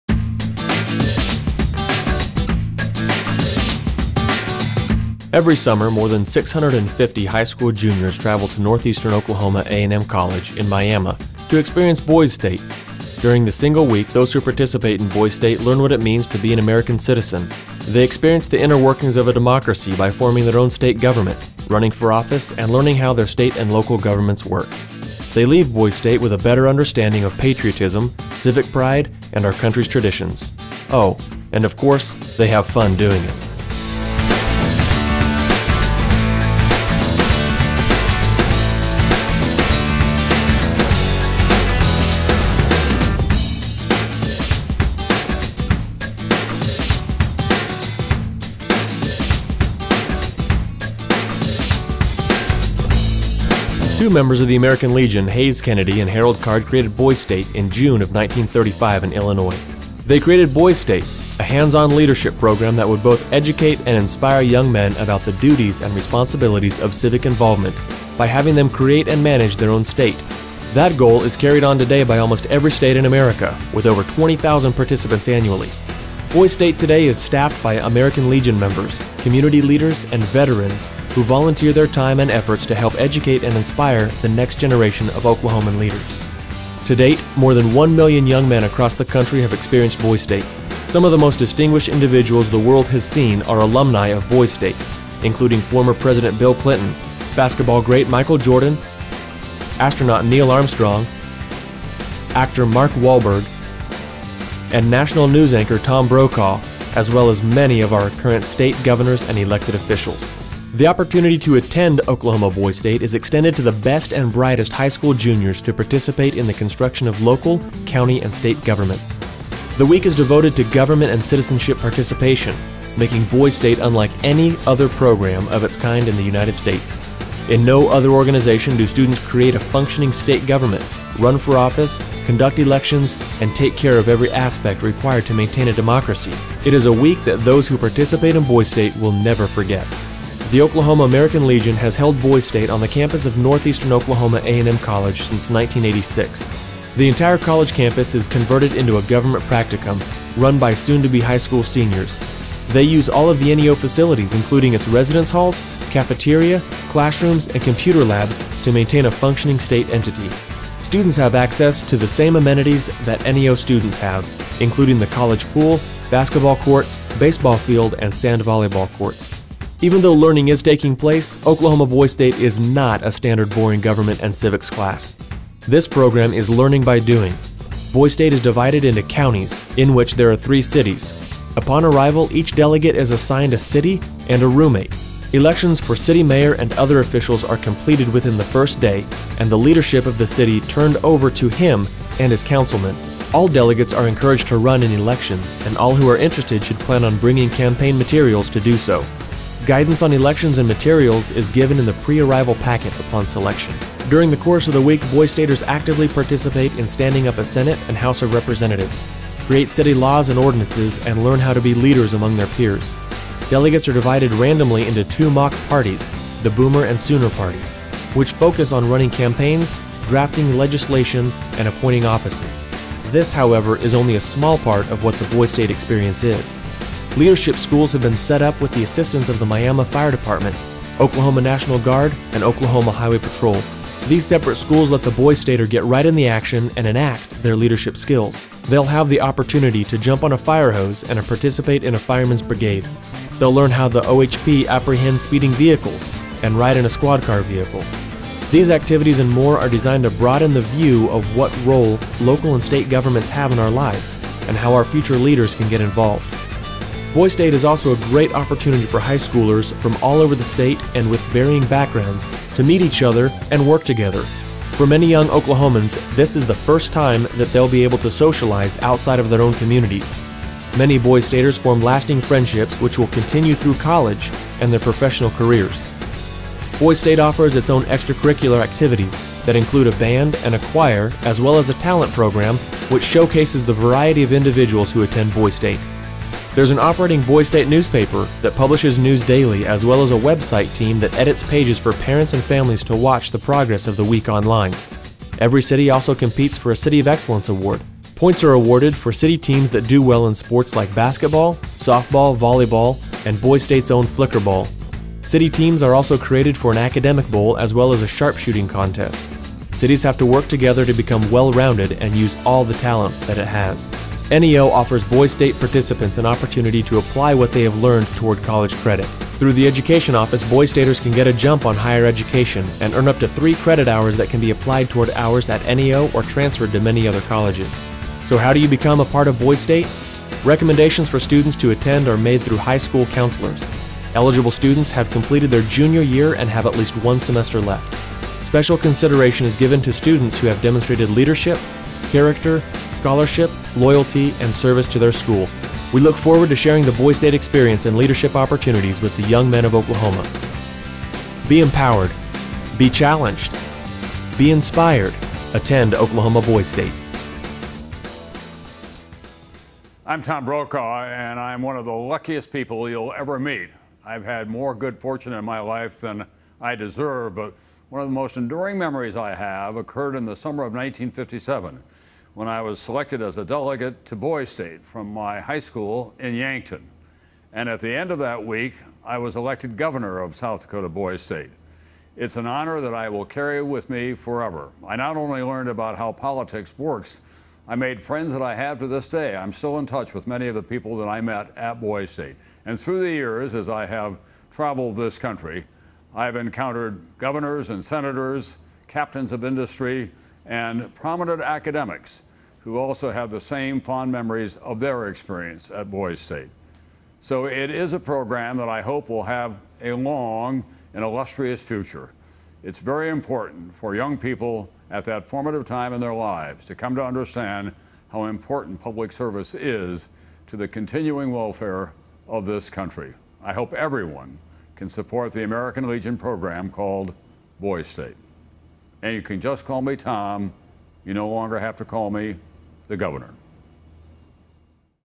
Oklahoma American Legion Boys State
The Oklahoma American Legion sponsors Boys State every year at NEO college in Miami, OK.  It is a government practicum that centers on patriotism and allows Oklahoma Youth to participate in the democratic process of our state and learn how to get involved in civic activity.
The program offers young Oklahomans to network with each other and prepare for life after high school.